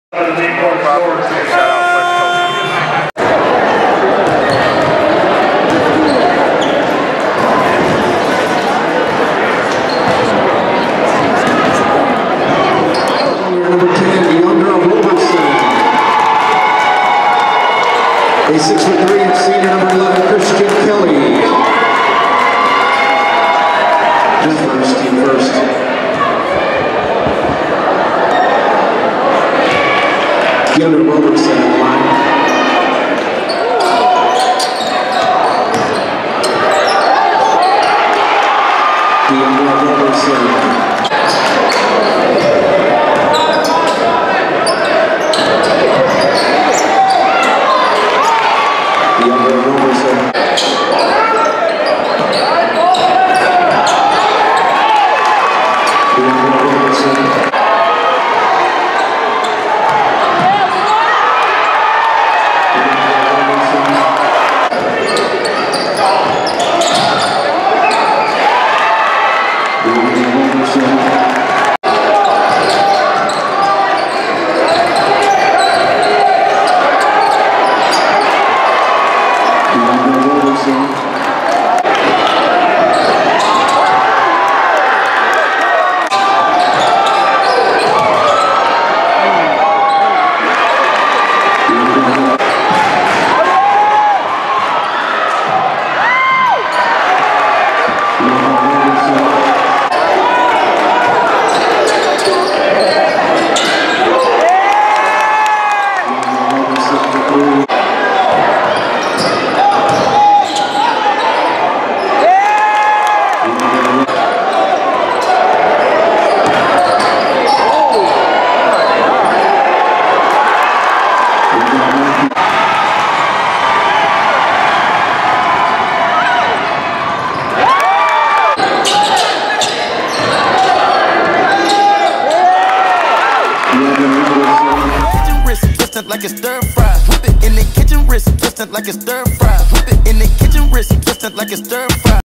Deundra Roberson Goes Off For 28 Points!!!
5’10 2018 PG Deundra Roberson dropped 28 points for Columbia Heights vs. top ranked DeLaSalle.